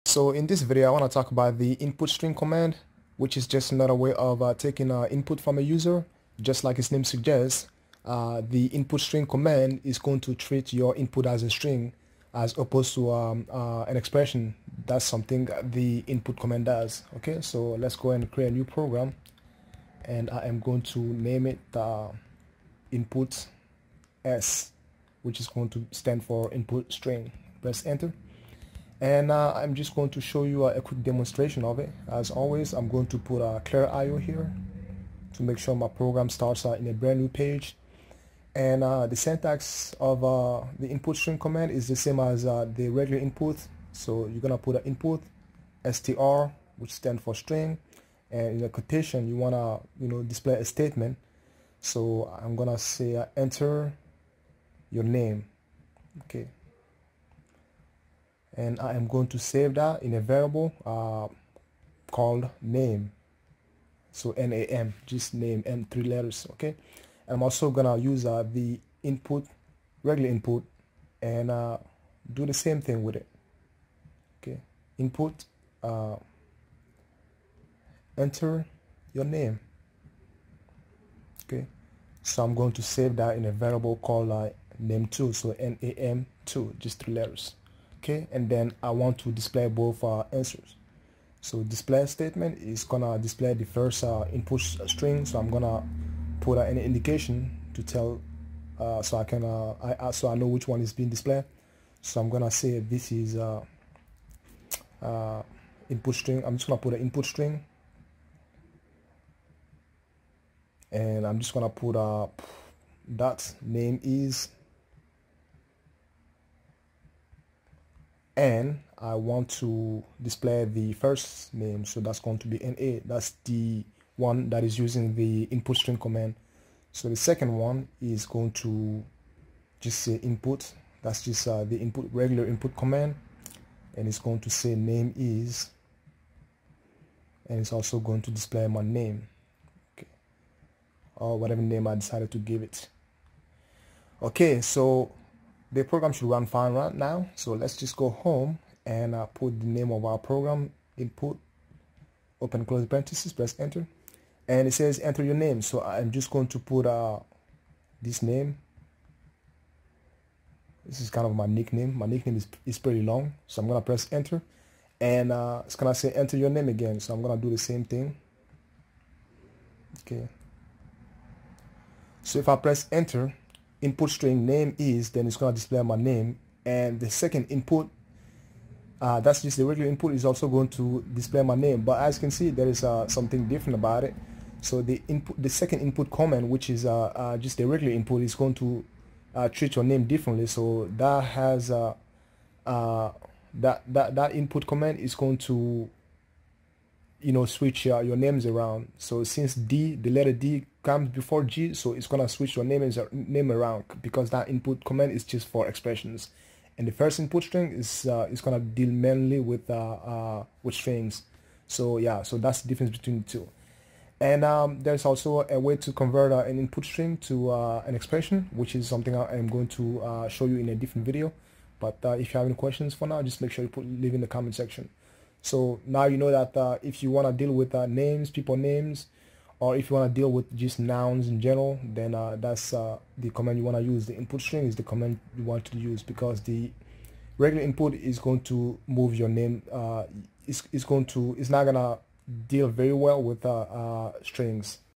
Programming a Texas Instruments - input VS inputStr (Ep 7)
Both commands take input from a user; one as an expression, the other as a string.